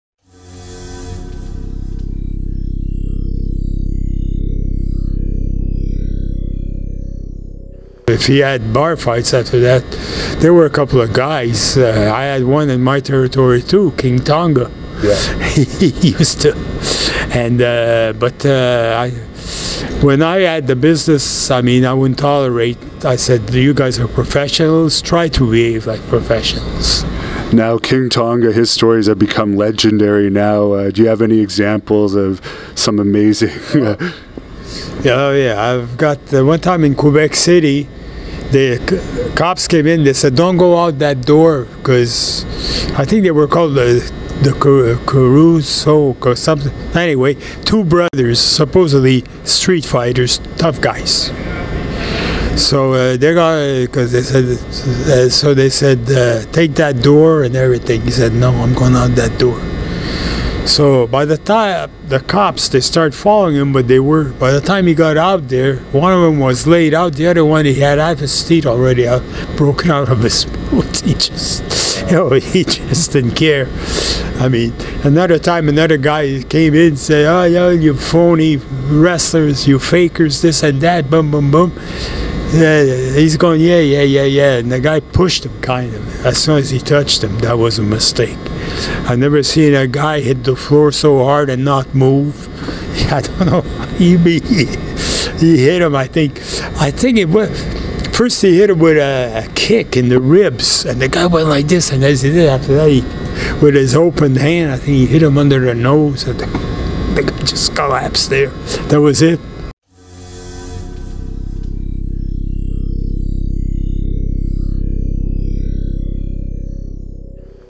Gino Brito on Haku's Toughness
Legendary Quebec wrestling promoter Gino Brito discusses the legendary toughness of Haku who wrestled for him in Quebec as King Tonga for years before going on to success in the WWE.